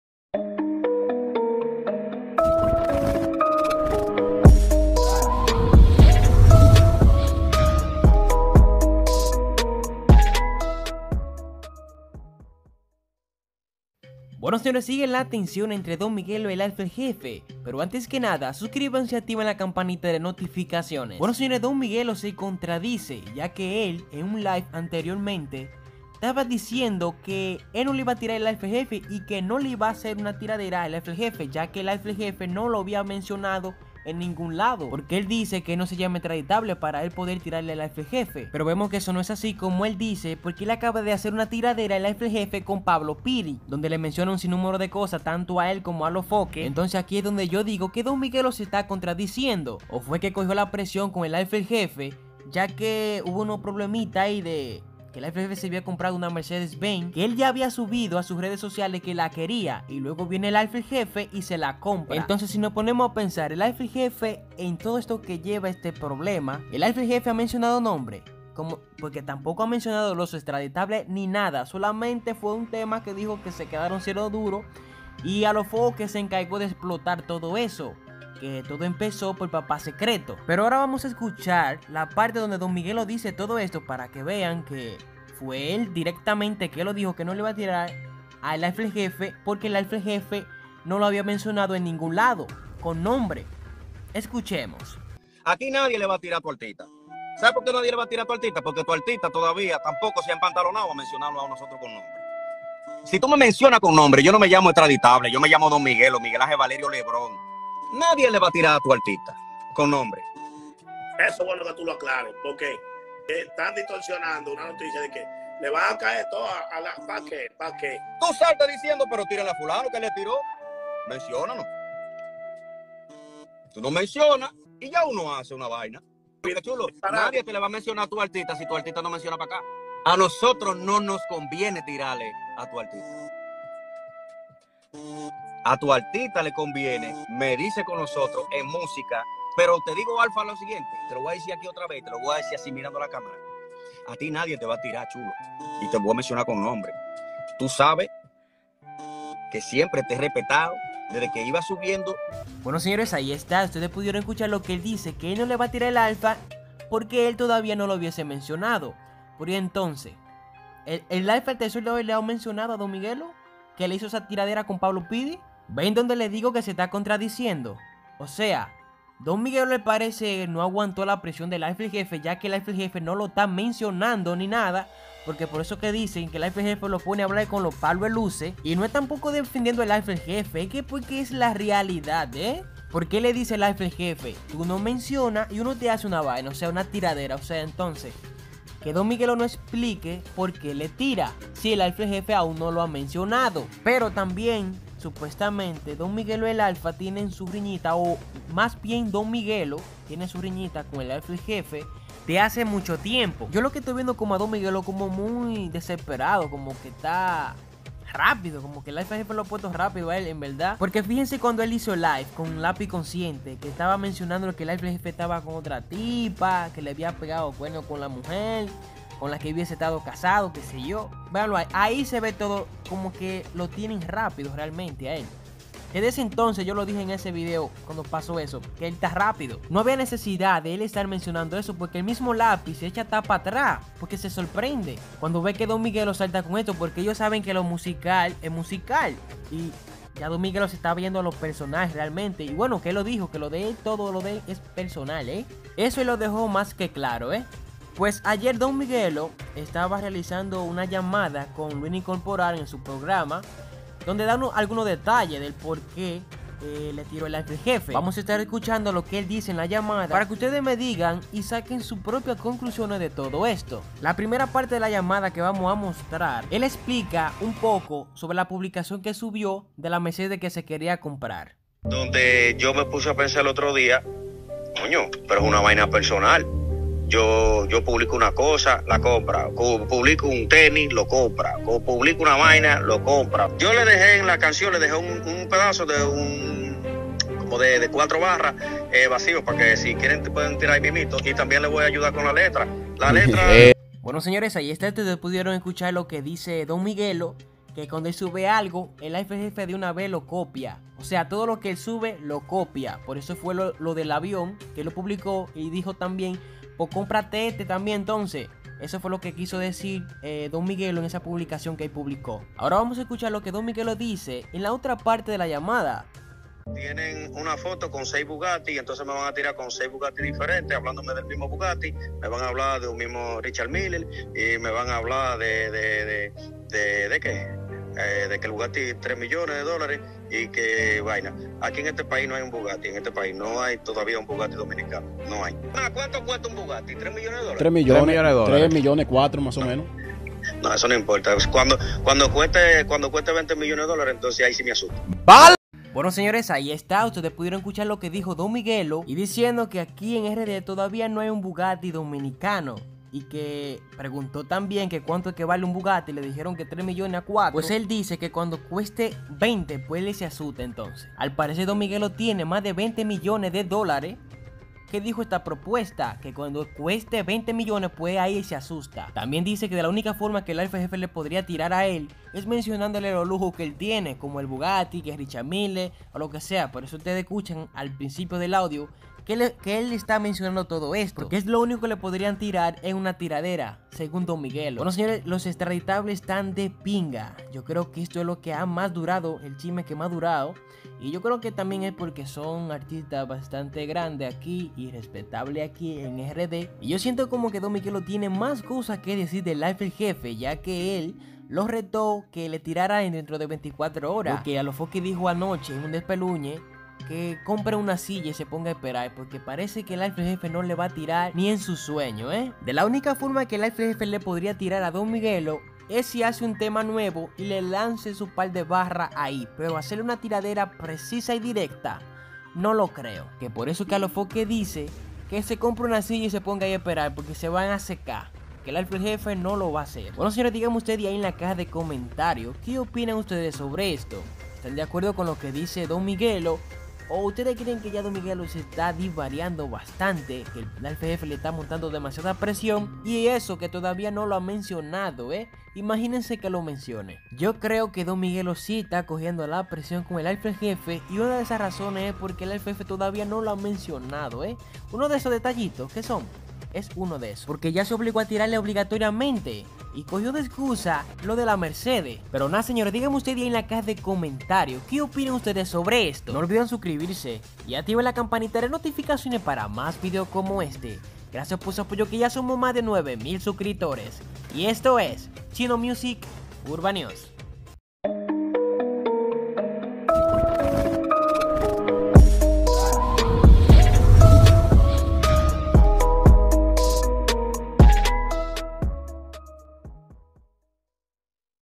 Don Miguelo EXPLOTA y Explica Por Que LE TIRO a El Alfa El Jefe y Alofoke
CM URBAN NEWS 👇👇❤